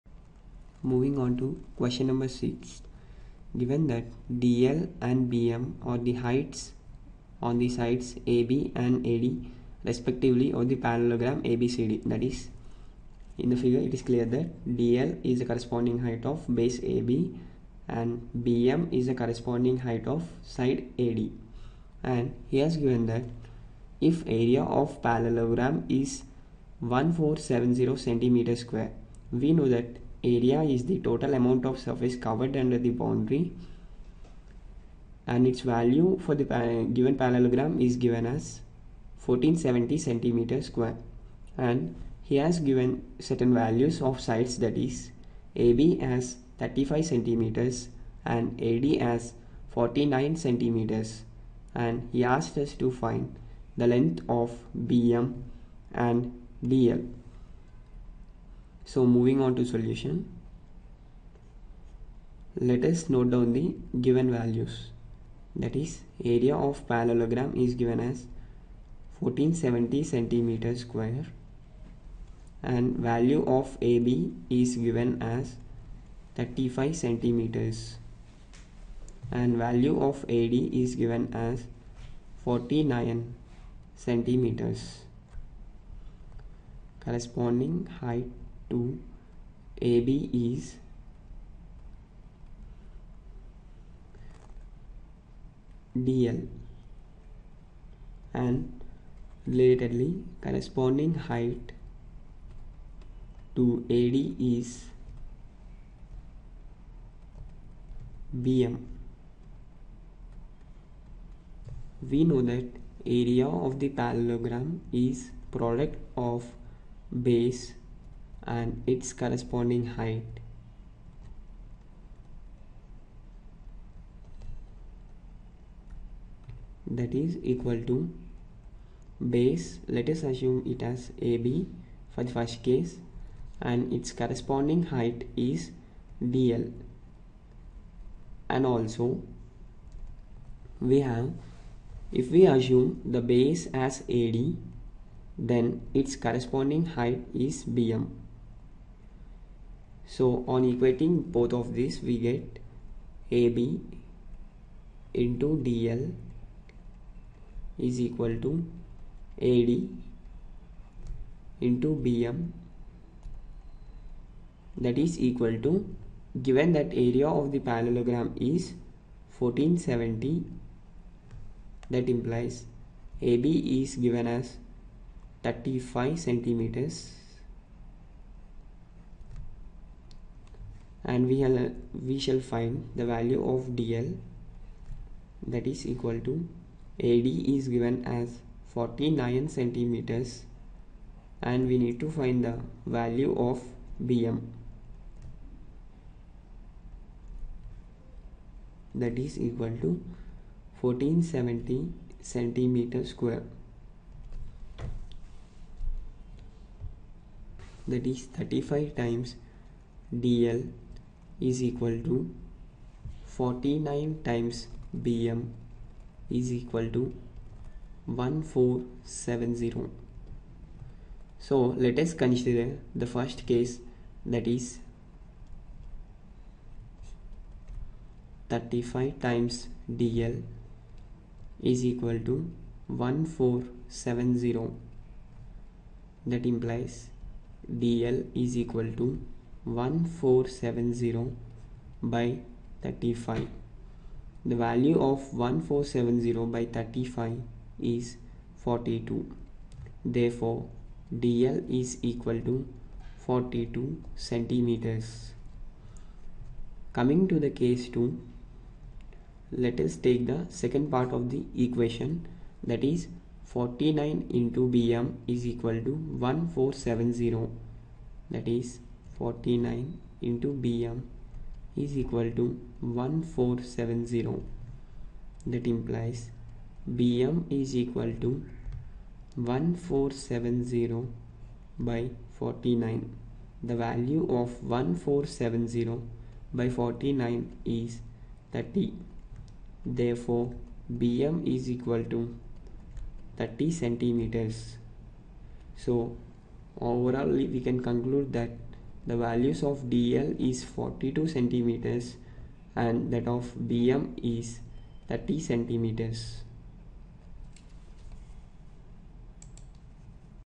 NCERT Solutions for Class 7 Maths Chapter 11 Perimeter and Area Ex11.2 Q6
1. Find the area of each of the following parallelograms